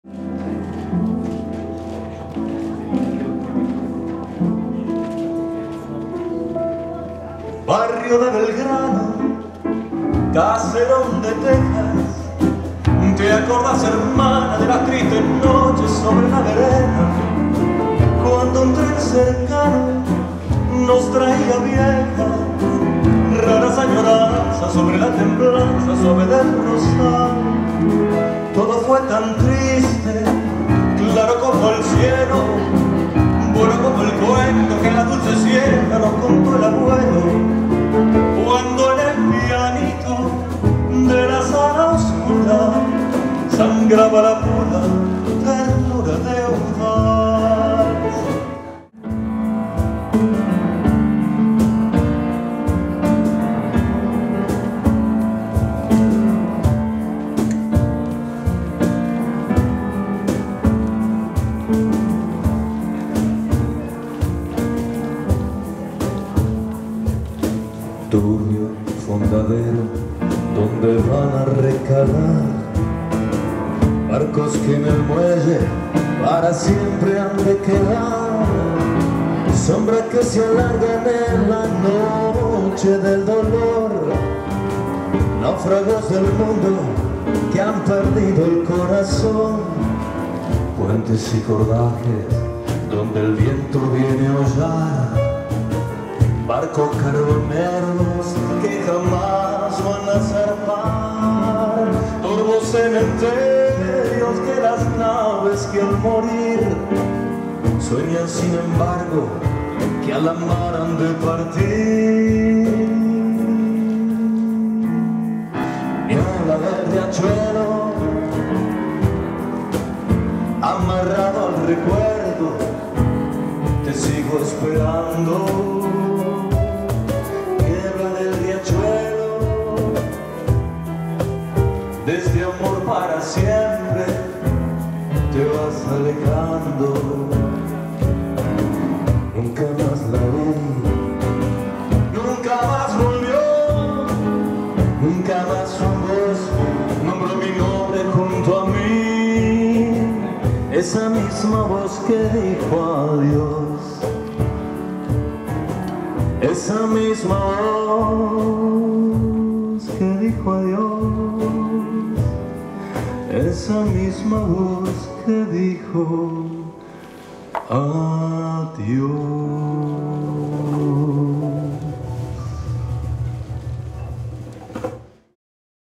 2 Tangos - with Sebastian Bossero, Singer
Lovely Tango Evening with: Sebastian Bossero, Singer- Javier Fernandez, Piano - Pablo Lacolla, Percussions in "El Social" July 2013 Zurich, Schweiz.